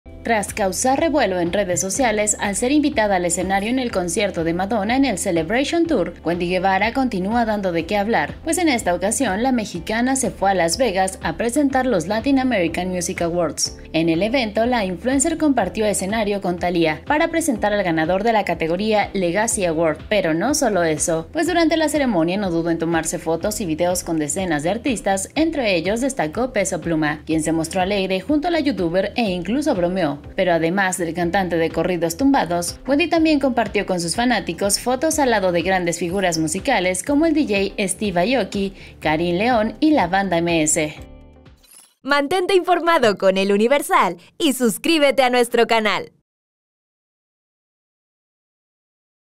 Wendy Guevara presume video junto a Peso Pluma
Wendy Guevara fue a Las Vegas a presentar los Latin American Music Awards